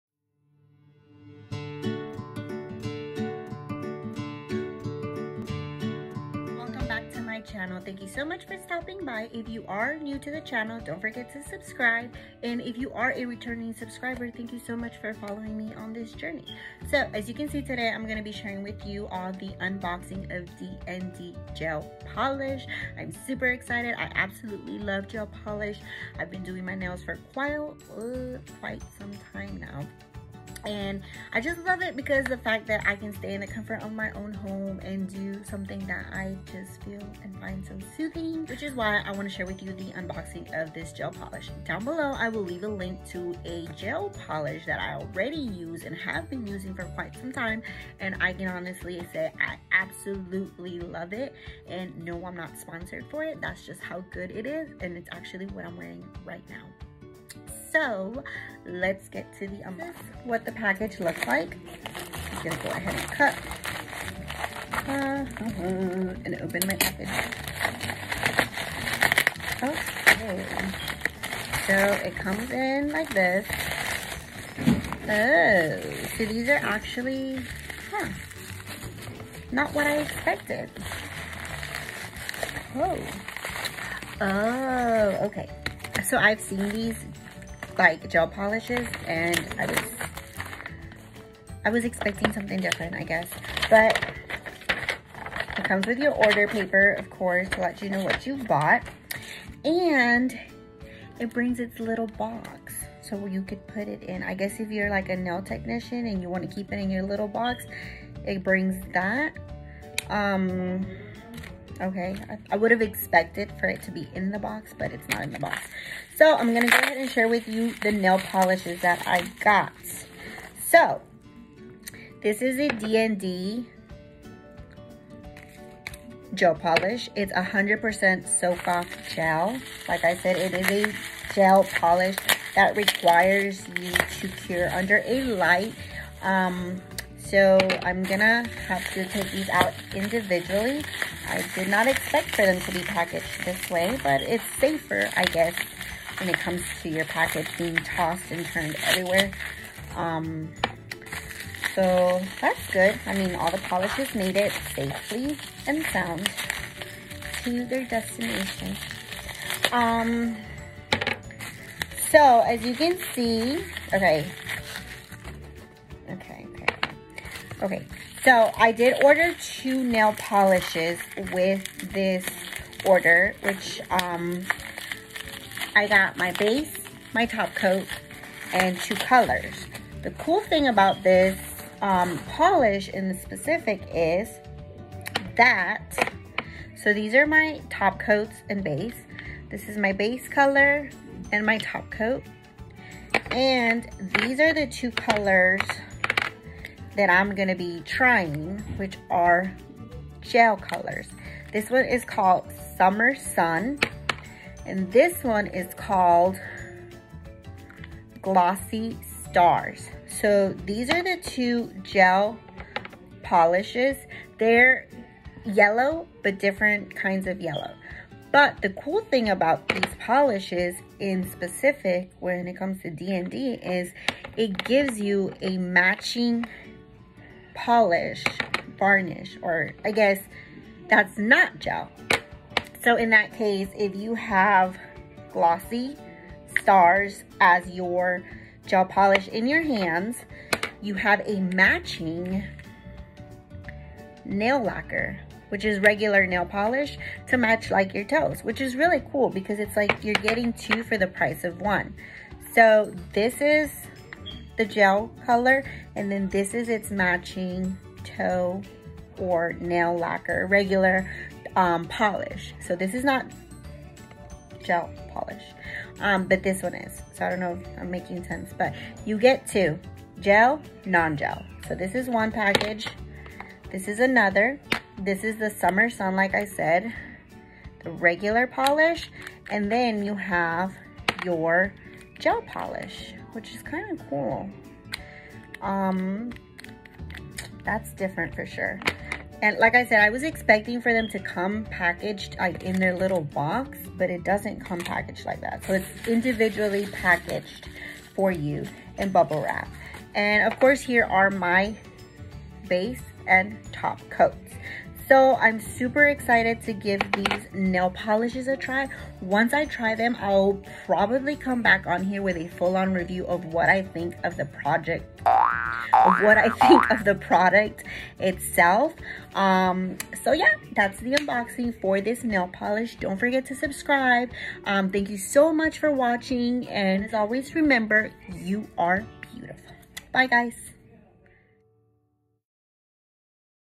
DND Gel Polish | Unboxing| $5.00 :o
Welcome back! Today I am unboxing my DND gel polish. This is my first time trying the product so i only ordered 2 gel polishes. If it all goes well i will return with more. I am super excited. So far i love the pricing. I will come back with a full review of the product. I hope you enjoy it as much as I do!! As always remember  you are beautiful. xoxo

Snap Irmalopez._
FB     Irma lopez